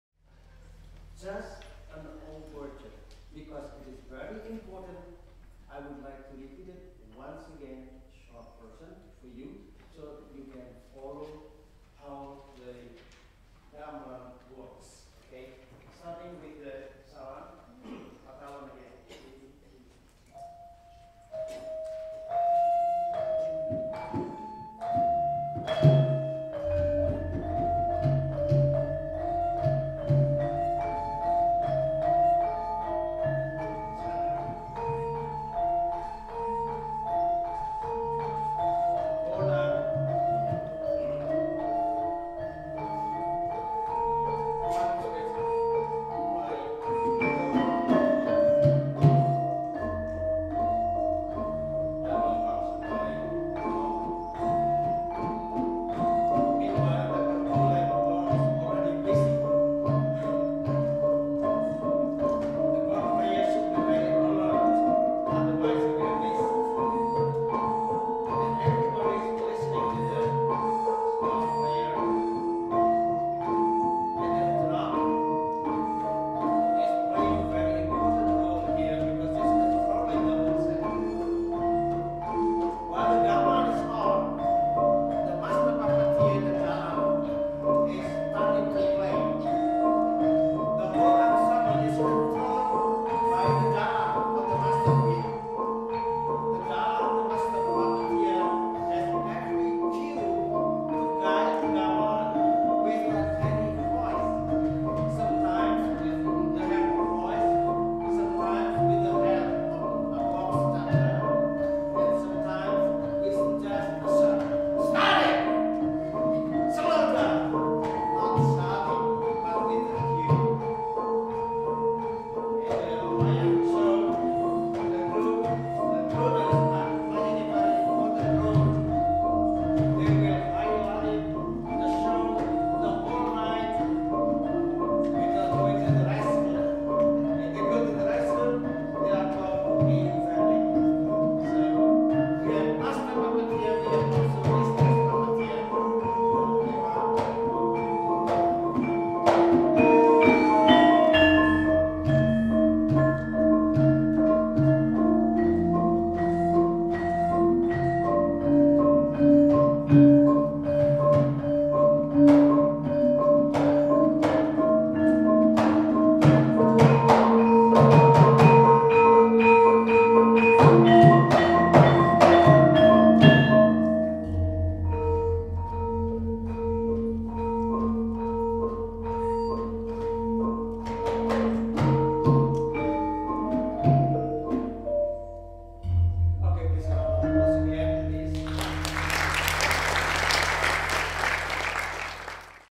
Indonesian Embassy Gamelan @ St. Mary's College of MD: PT1
The Indonesian Embassy Gamelan performing with Santi Budaya at St. Mary's College of Maryland.

I played Slenthem; this was the first time I performed with a gamelan and the fourth or sixth time I had ever playedat all.